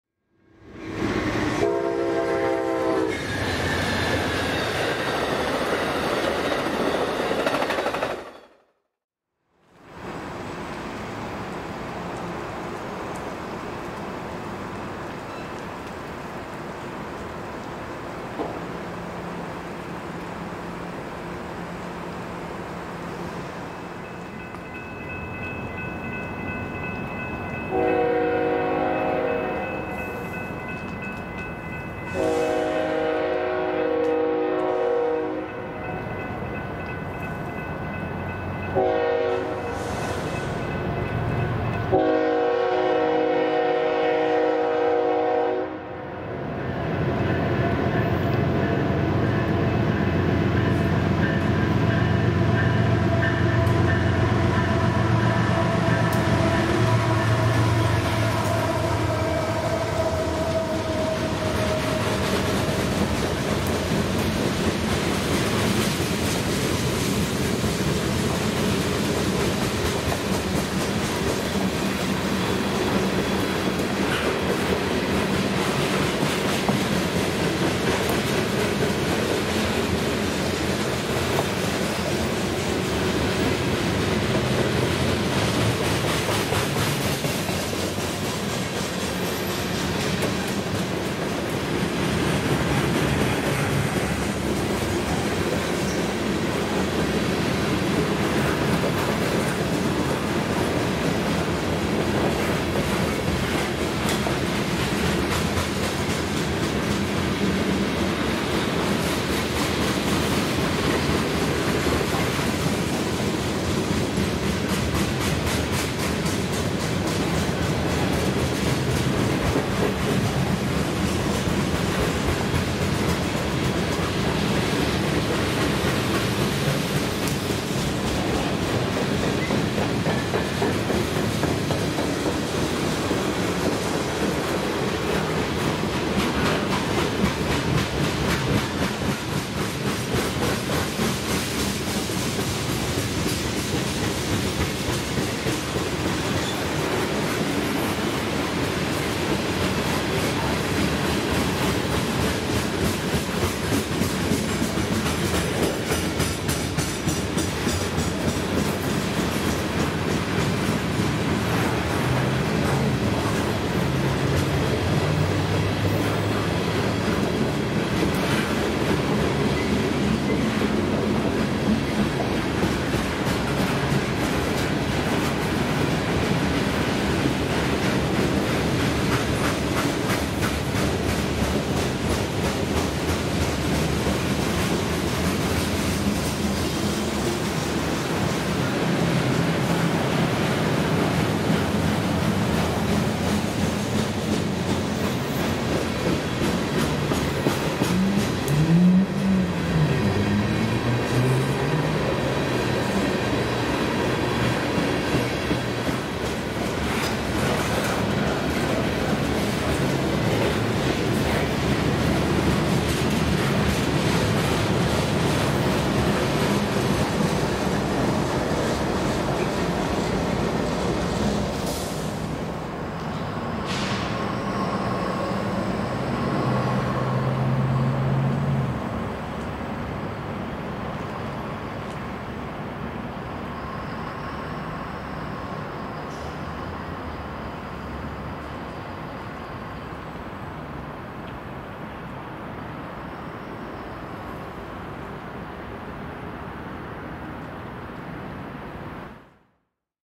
BNSF Freight Train Rolls Through Auburn
On a foggy November day, a BNSF freight train rolls through the Auburn Sounder Station with a mix of boxcars, hoppers, center beams, and tank cars.

This was filmed on the BNSF Seattle Subdivision in Auburn, Washington. The rail line is used by Sounder and Amtrak passenger trains along with BNSF freight trains on a daily basis.

Originally recorded on November 27th, 2023.